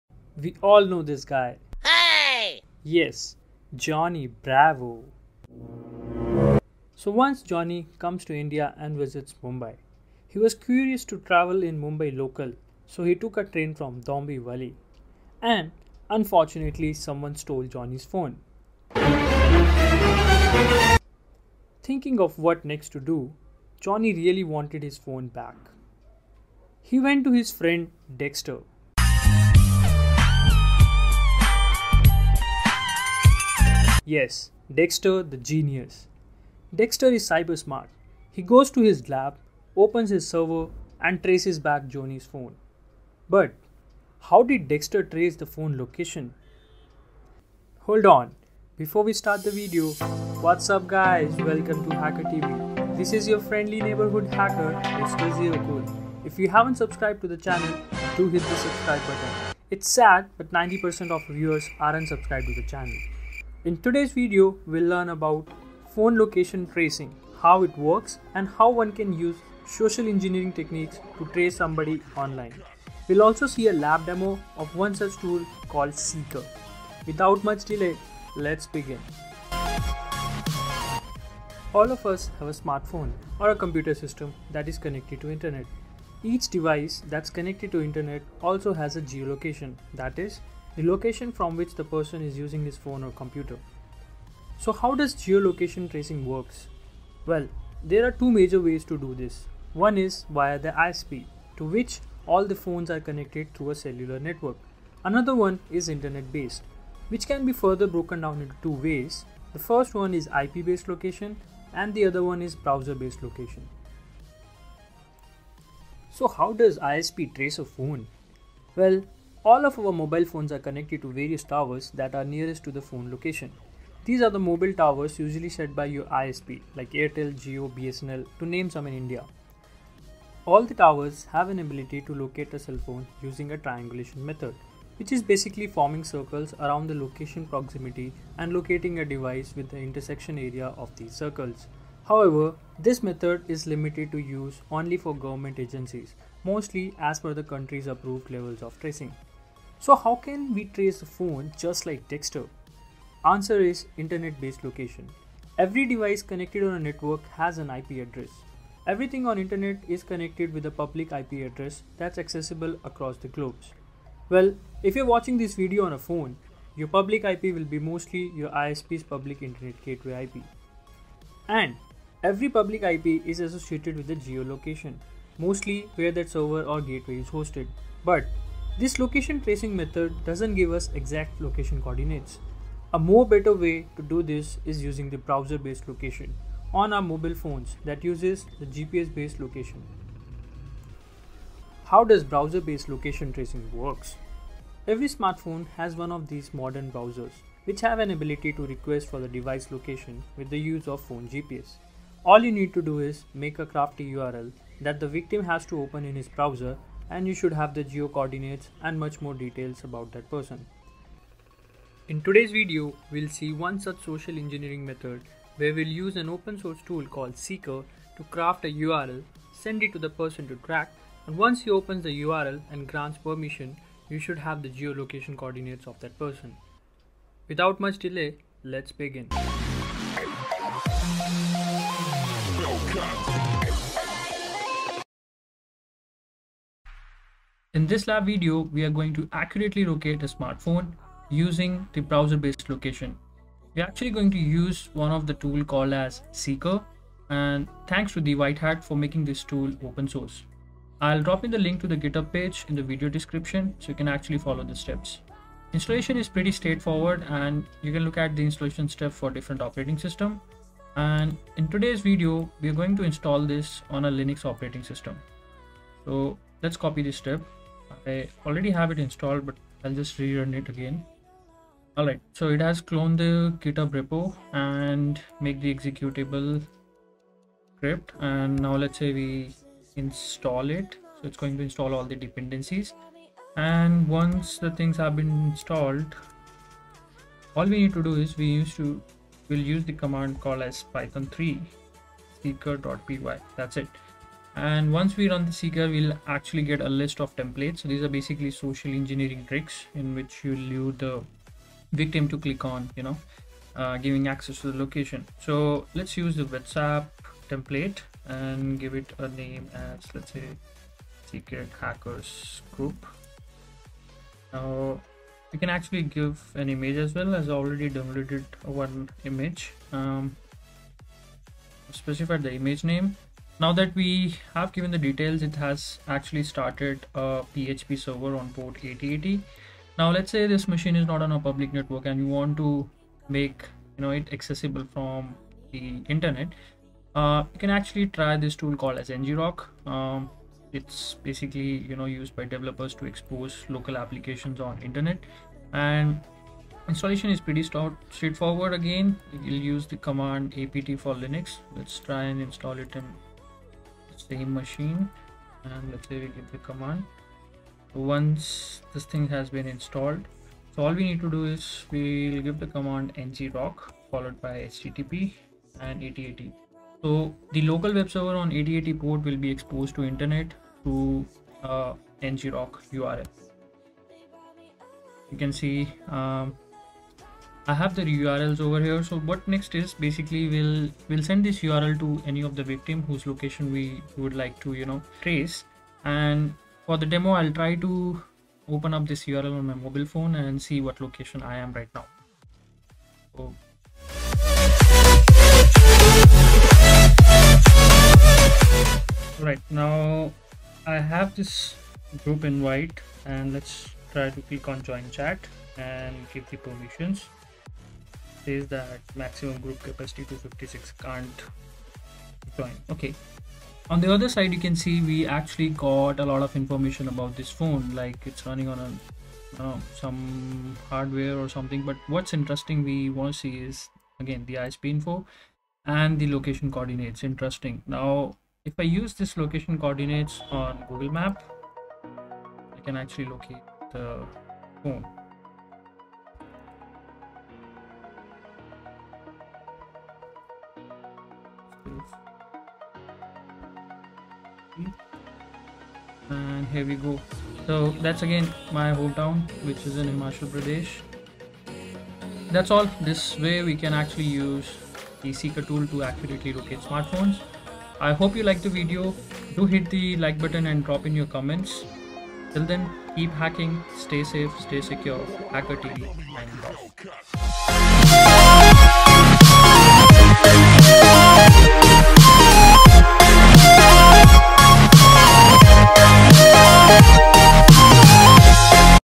Phone Location Tracing Explained Learn with live smartphone geolocation tracking
This video is about How Phone Location tracing Works - What are different methods to locate/trace a phone. How do ISPs and government trace a phone? How can someone leverage the internet to trace a criminal? Video also has a lab-based demonstration to show how we can use Seeker Tool to accurately locate smartphones using social engineering methods.

How did I make this video?